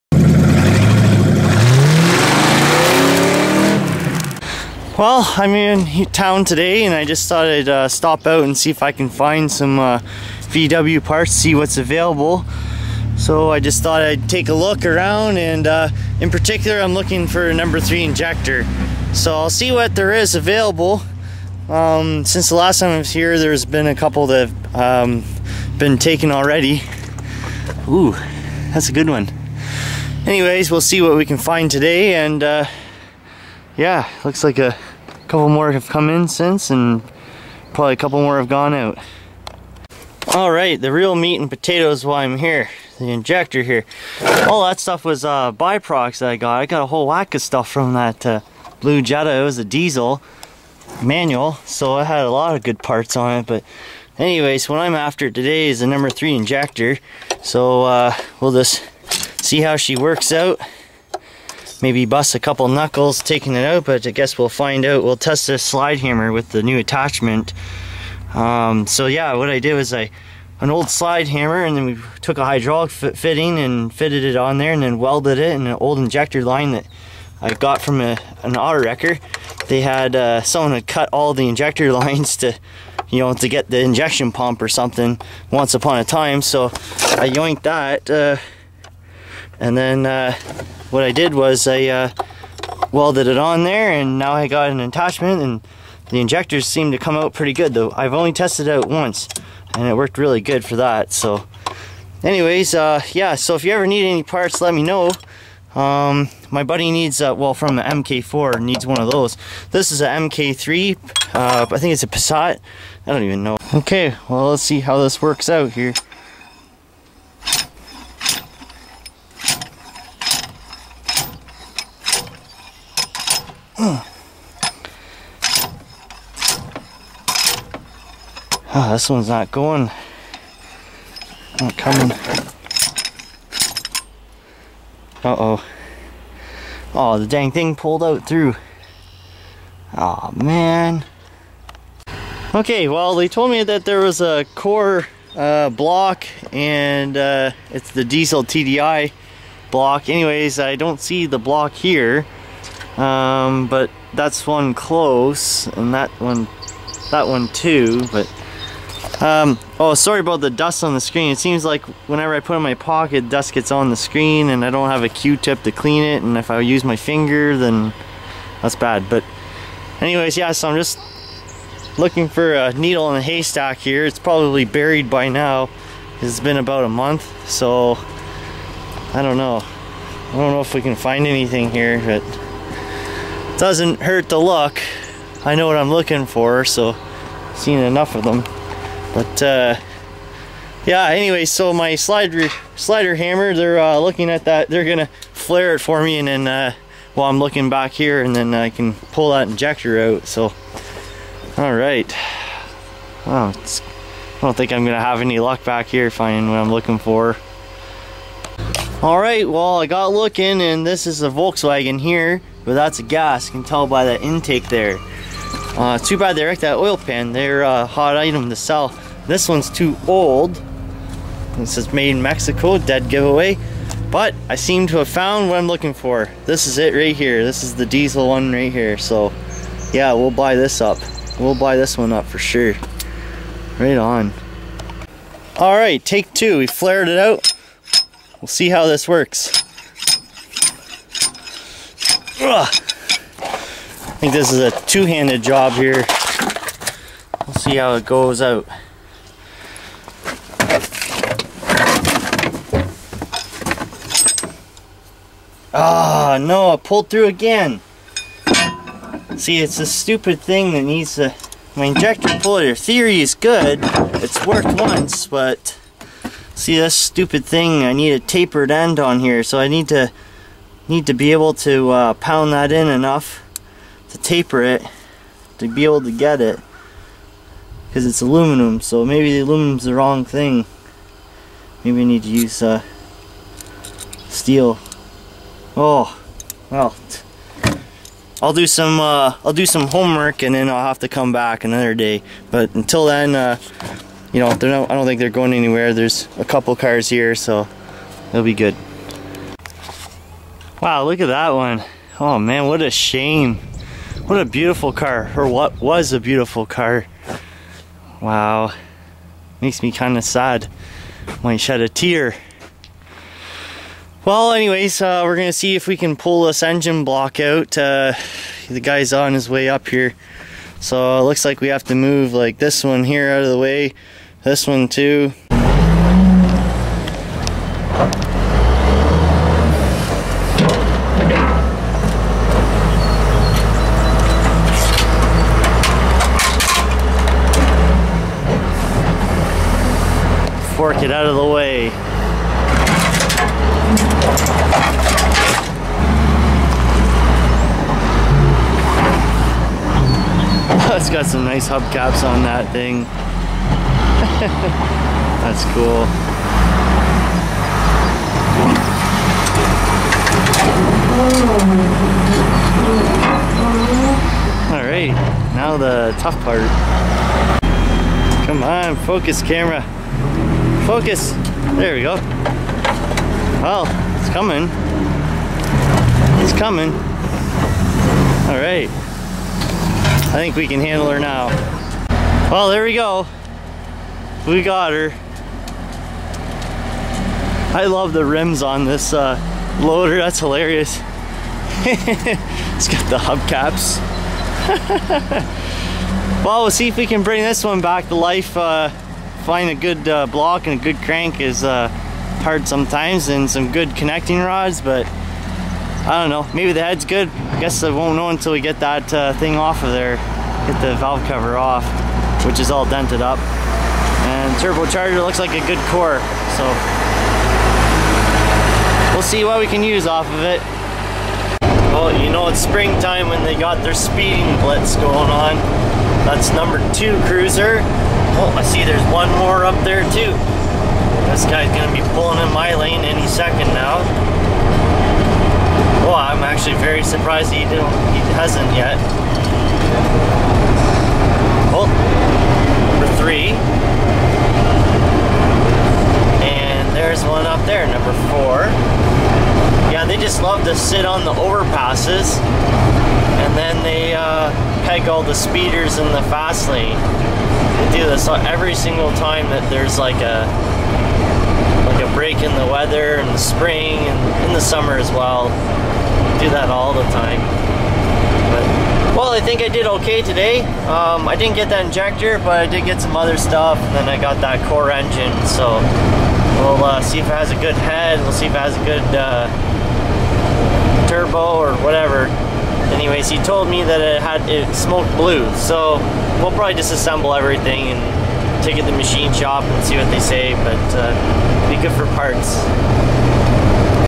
Auto Wrecker Part Harvest
I just love to go on a treasure hunt. I broke my prototype!